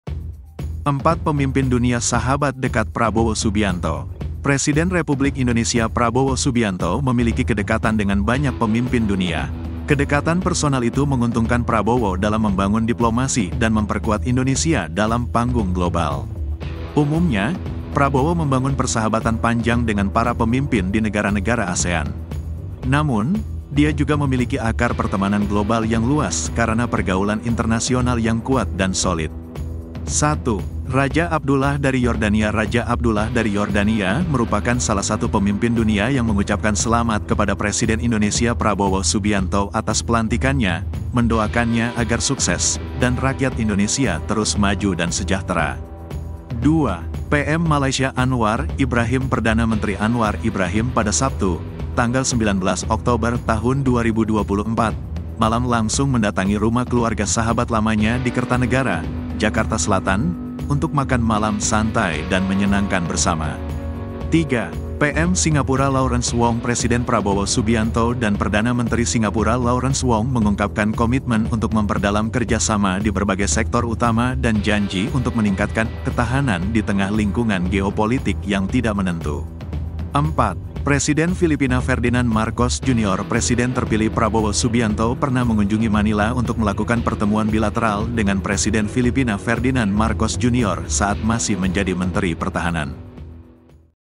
4 Pemimpin Dunia Sahabat Dekat Prabowo Subianto
Presiden Republik Indonesia Prabowo Subianto memiliki
kedekatan dengan banyak pemimpin dunia. Kedekatan
personal itu menguntungkan Prabowo dalam membangun
diplomasi dan memperkuat Indonesia dalam panggung global.
Editor Video: Adi Maulana